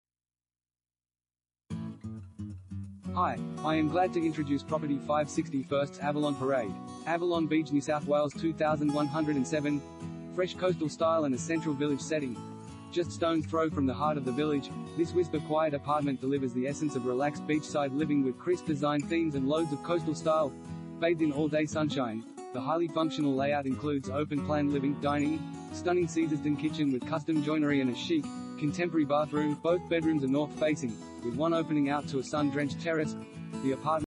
5/61 Avalon Parade, Avalon Beach NSW 2107 | AuPropertyReport.Com
5/61 Avalon Parade, Avalon Beach NSW 2107
Property Type: unit
Bedrooms: 2
Bathrooms: 2
Car Space: 1
Fresh coastal style and a central village setting